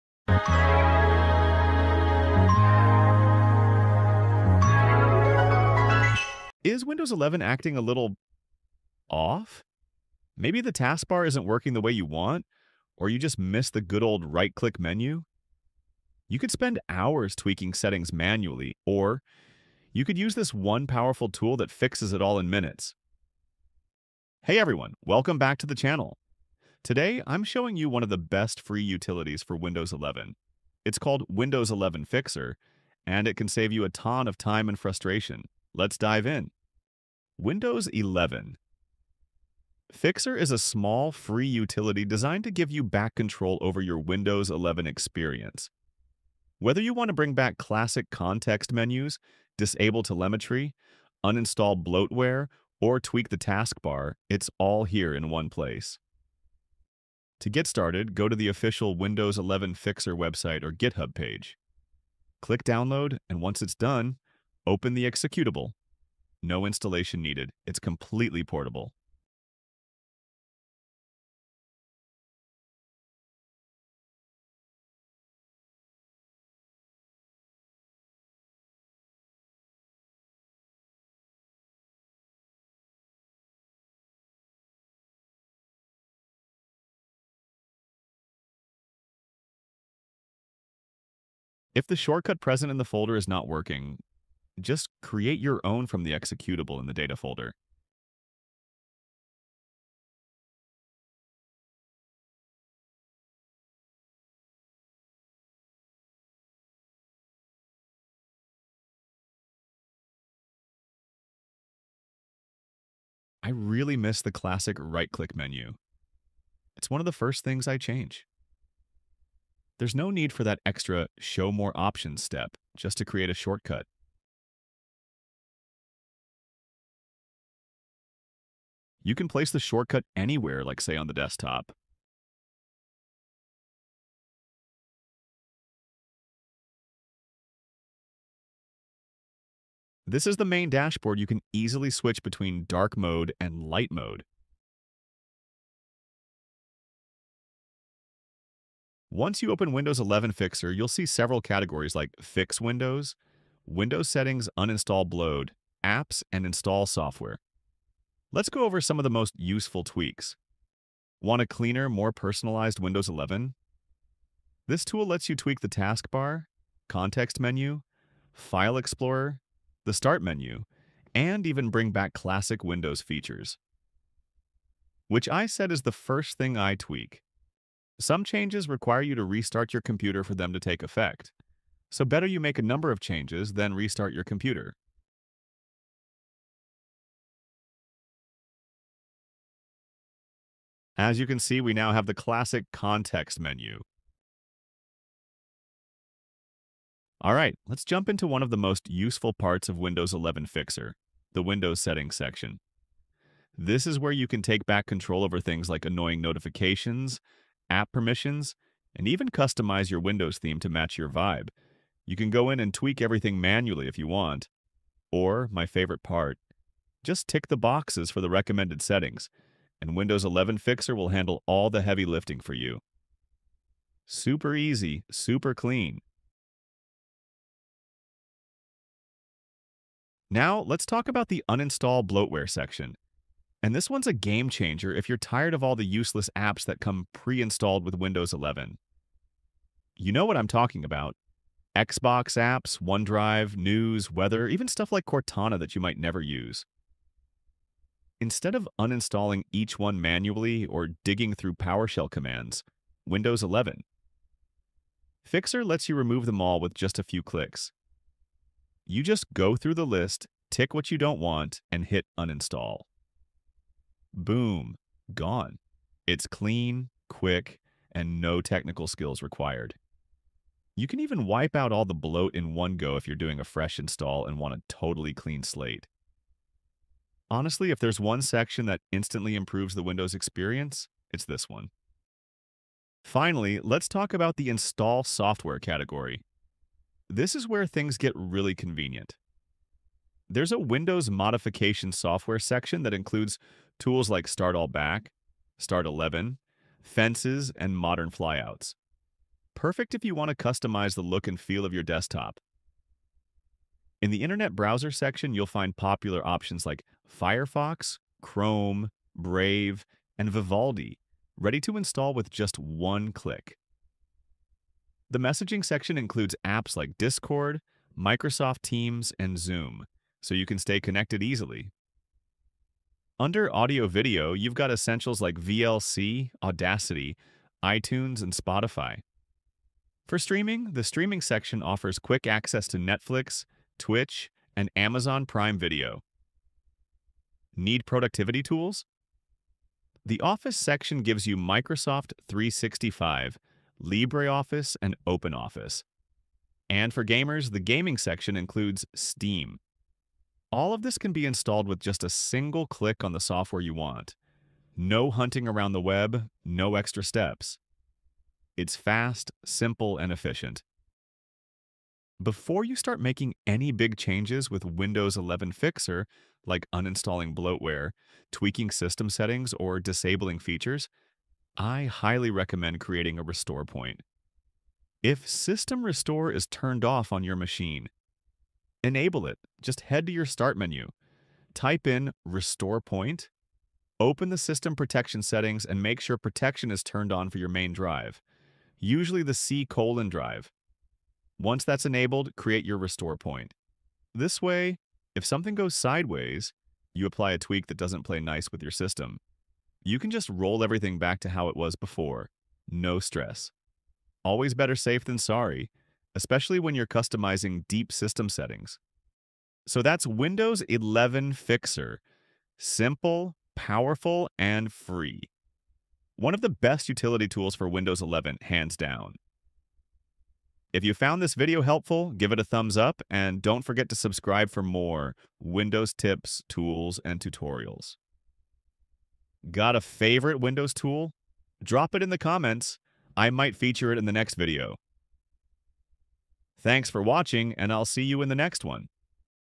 Best Free Utility Tool for Windows 11 – Windows 11 Fixer
If you’ve just installed Windows 11 or upgraded and things don’t feel quite right, this is the tool you’ve been waiting for. In this video, I’ll show you how to use Windows 11 Fixer to optimize performance, remove bloatware, customize the taskbar, bring back classic menus, improve privacy settings, and much more—all in just a few clicks.

🛠️ Whether you're a beginner or a power user, this tool makes Windows 11 work the way you want it to.
✅ Fast
✅ Safe
✅ No registry hacks
✅ No command line needed